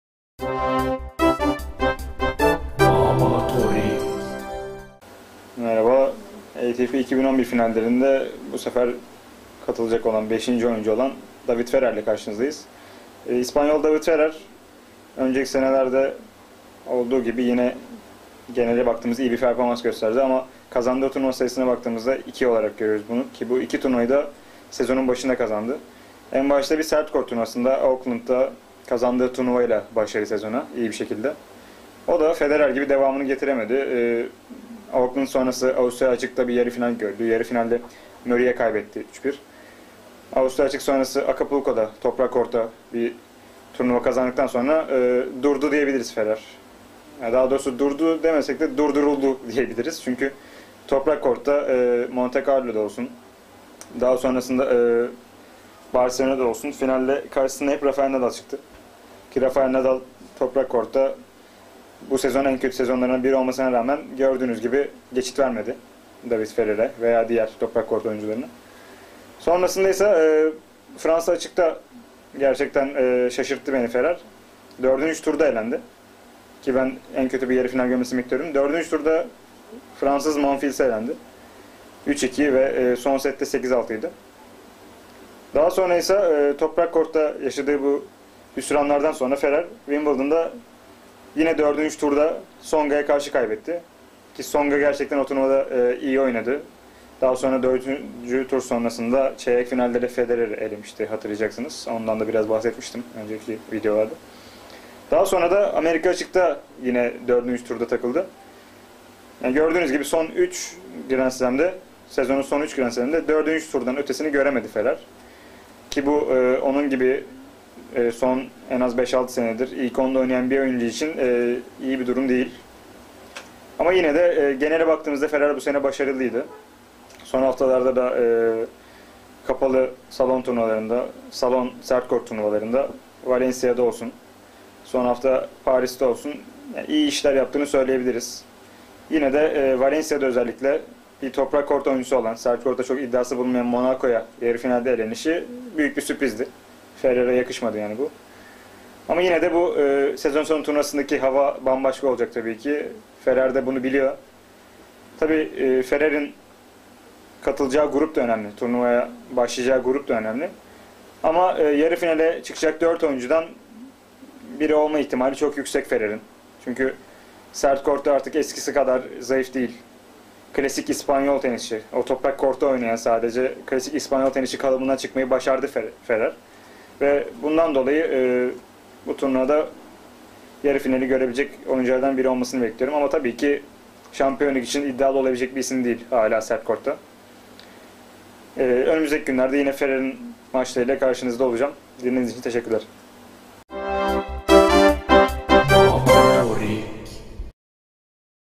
Babatorik ATP 2011 Finalleri Oncesi David Ferrer
barkin anlatiyor 14 Kasim 2011 ATP 2011 Finalleri Oncesi David Ferrer
Ferrer son dordun guclu adaylarindan